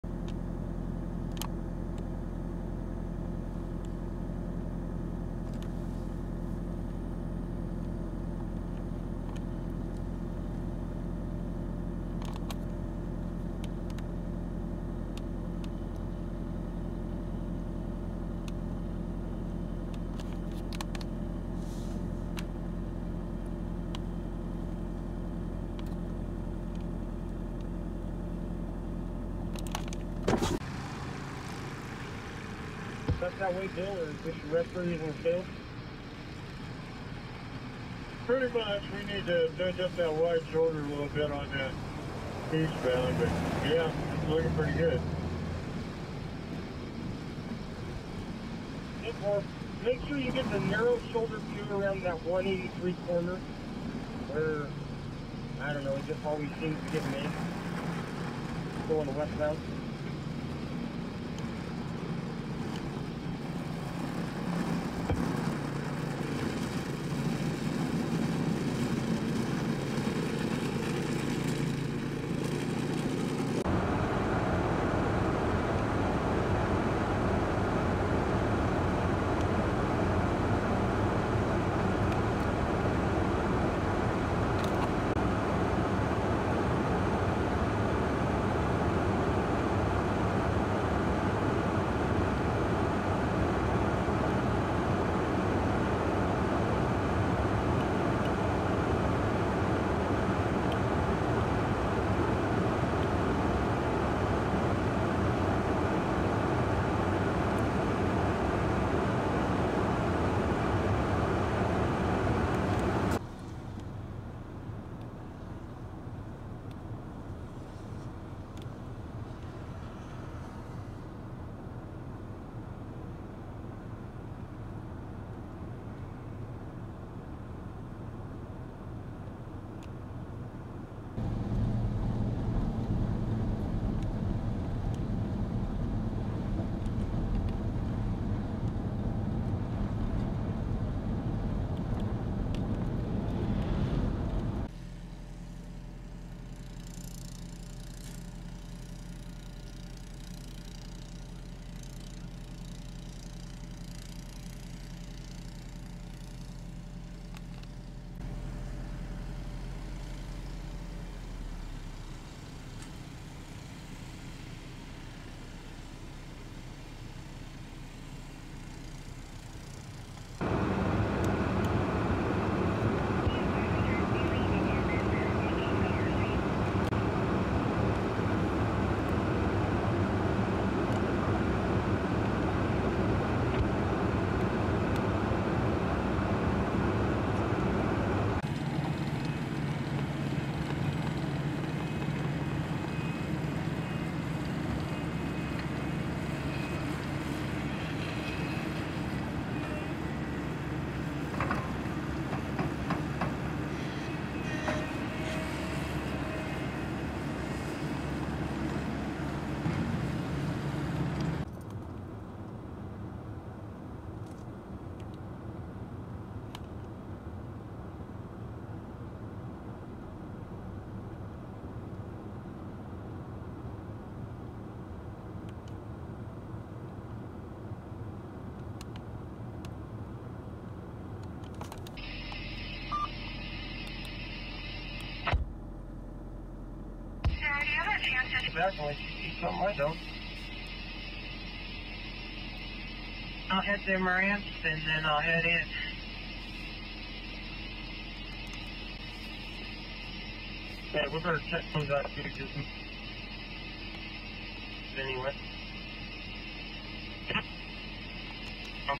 11-25-2023 North Platte, NE - Digging Out of Snow
After the first significant snowfall of the year for the North Platte area, many residents can be seen trying to dig their way and clear over 5 inches of snow. Many people were using snow blowers and shovels while some businesses utilized trucks with a plow on the front.